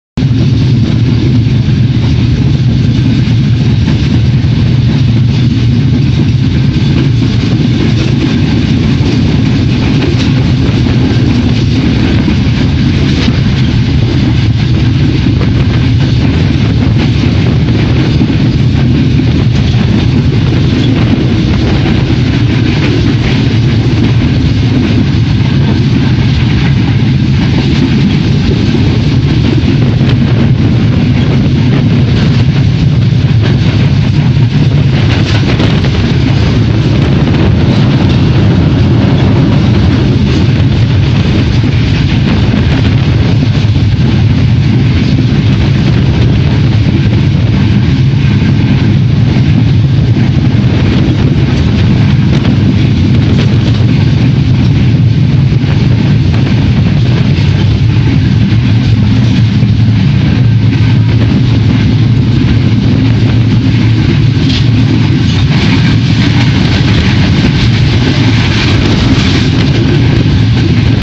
Traveling by train in Sri Lanka
Brown Traveller travelling in train in Sri Lanka from Colombo to Kandy in 3rd class.Follow me at browntraveller.com and twitter: @brown_traveller